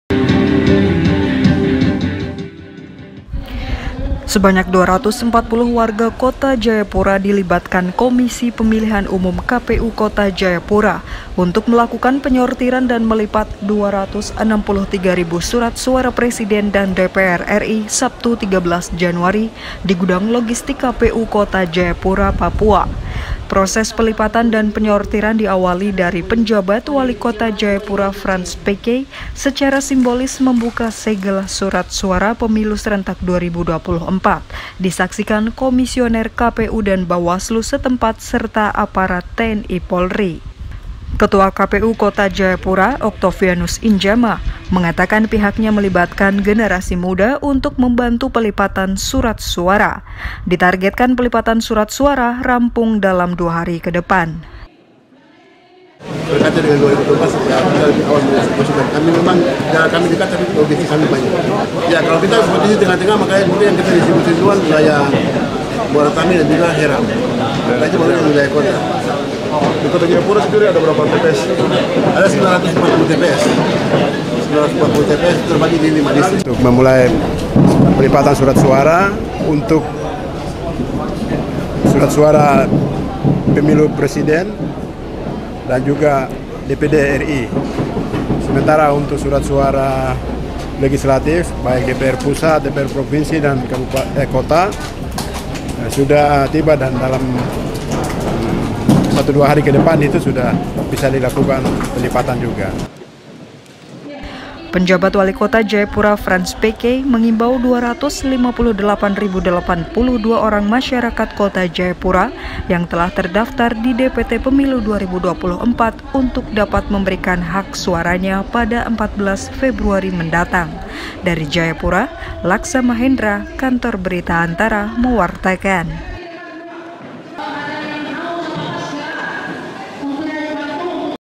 KPU Kota Jayapura Libatkan 240 Warga Lipat Surat Suara Pemilu 2024
ANTARA - Komisi Pemilihan Umum (KPU) Kota Jayapura melibatkan 240 warga yang didominasi oleh generasi muda untuk melakukan penyortiran dan melipat 263 ribu surat suara Presiden dan DPD RI Pemilihan Umum (Pemilu) 2024 di gudang logistik KPU setempat. Ketua KPU Kota Jayapura Oktovianus Injama menargetkan pelipatan suara rampung dalam dua hari ke depan. (Laksa Mahendra/Soni Namura/Rijalul Vikry)